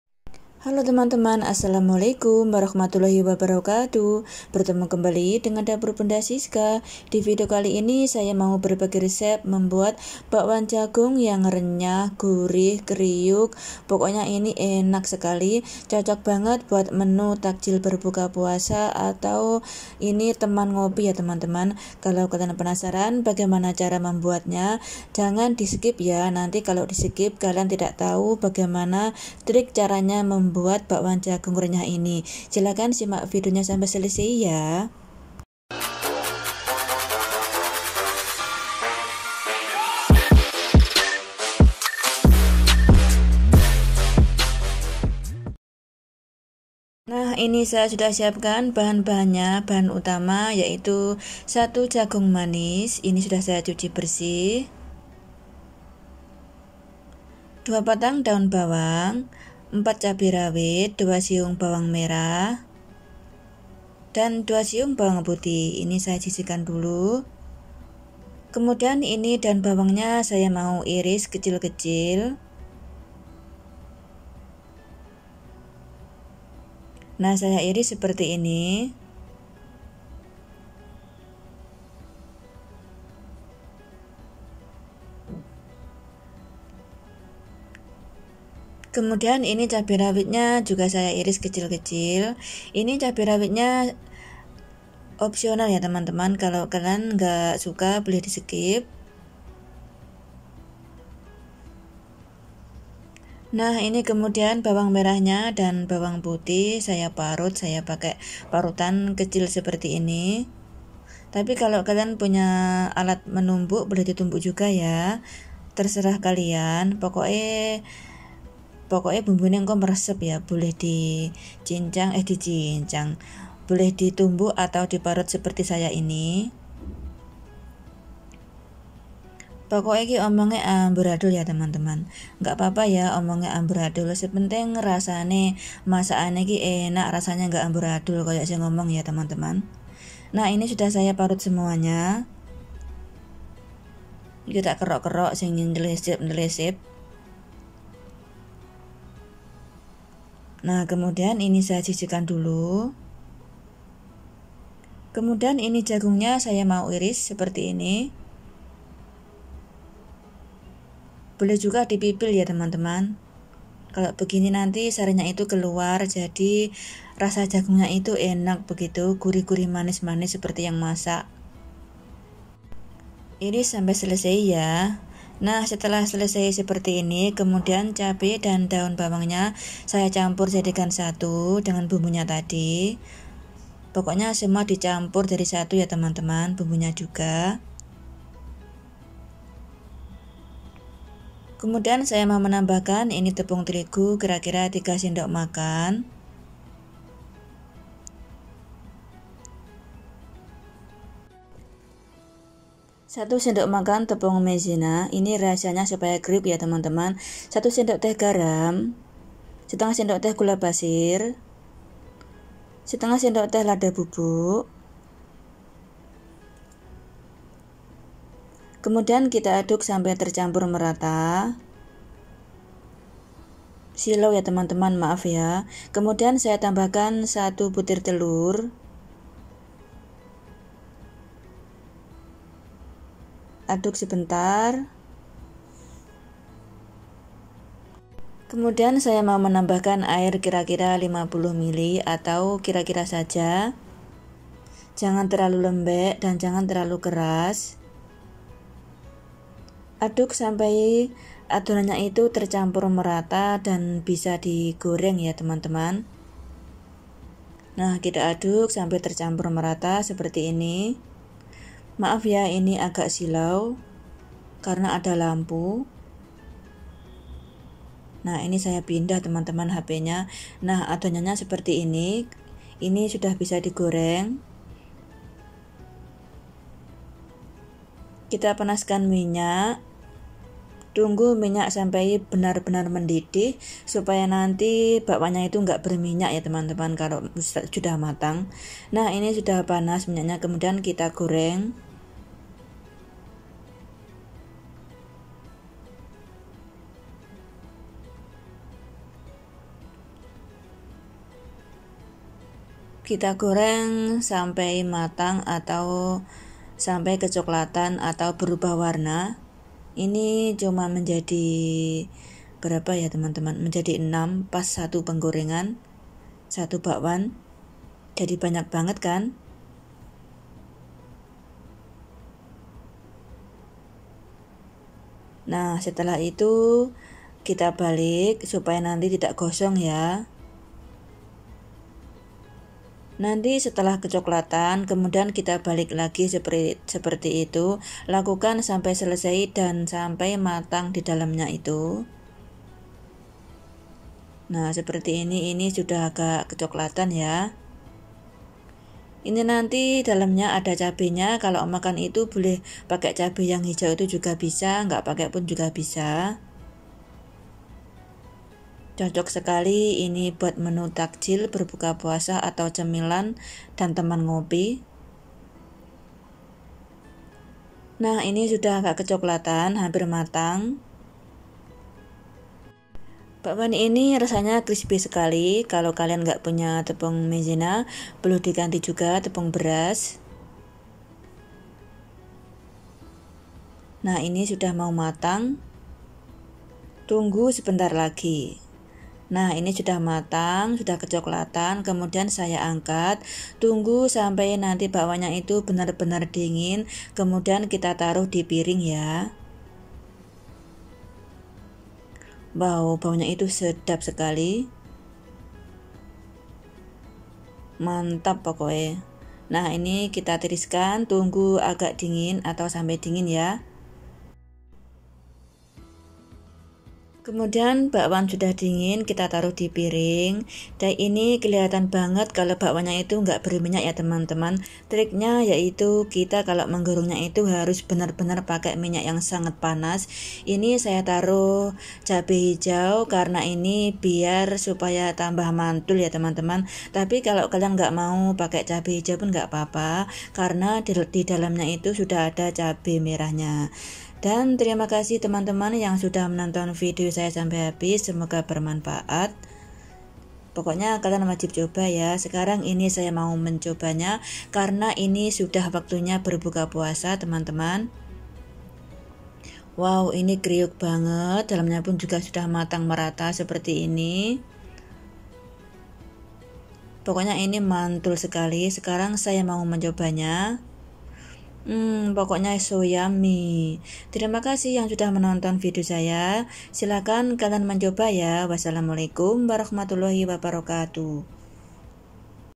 trik dan cara supaya bakwan jagung kalian renyah, crispy, tahan lama dan tidak lembek/berminyak
BAHAN
-1 jagung manis
-3 sdm tepung teligu
-1 sdm tepung mezena
-2 batang Daung bawang
-4 cabe rawit
-2 siung bawang putih
-2 siung bawang merah
-1 butir telur
-50 ml air bersih
-1 sdt garam
-1/2 sdt gula pasir
-1/2 sdt Lada bubuk

Lihat video sebelumya
Rahasianya membuatwil